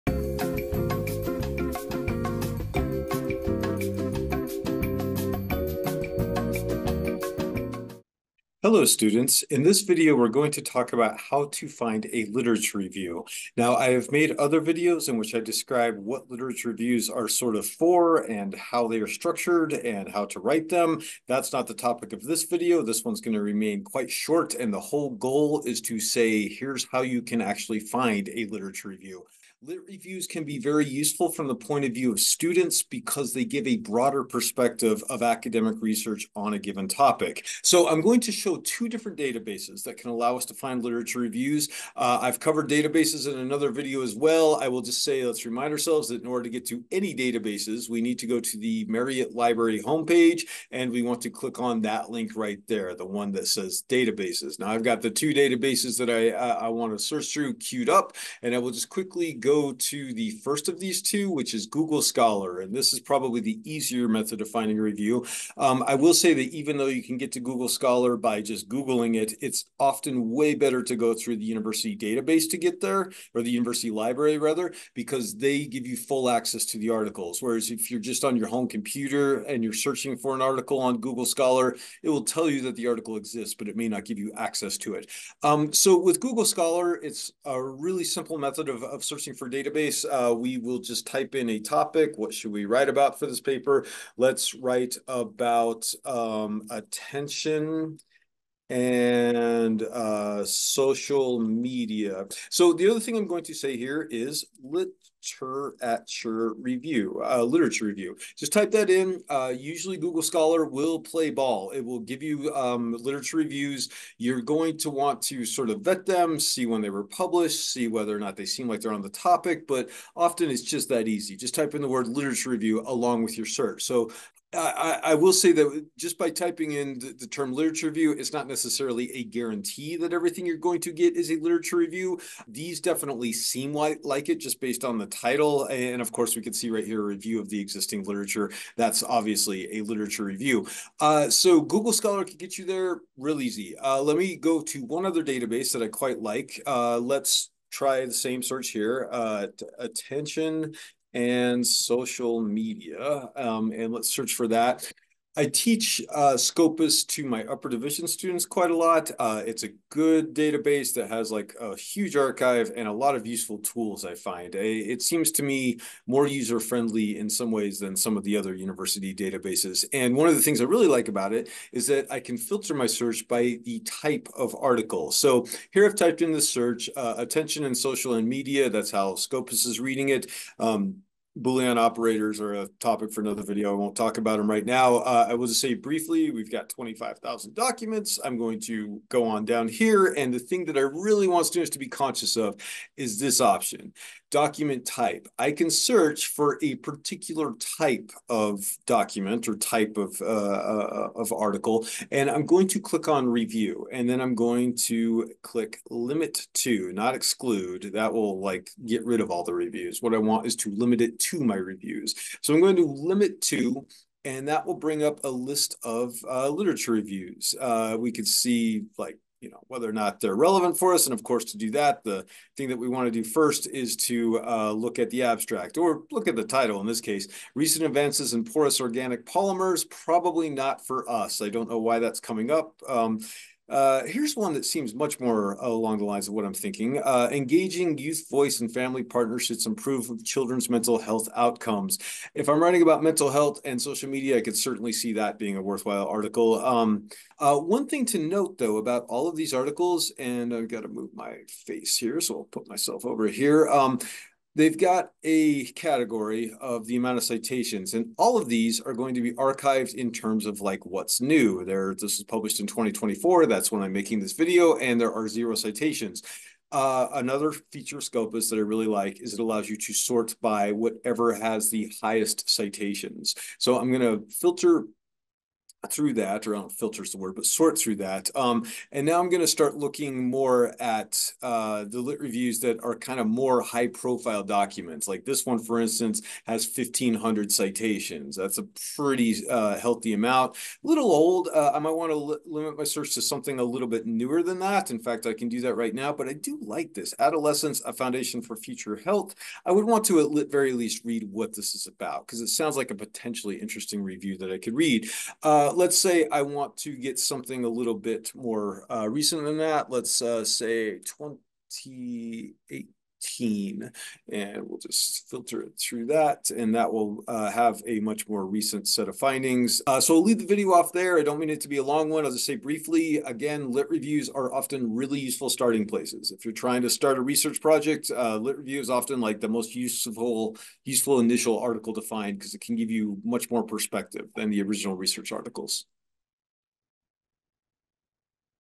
How to find literature reviews
This video describes how to find a literature review using Google Scholar or Scopus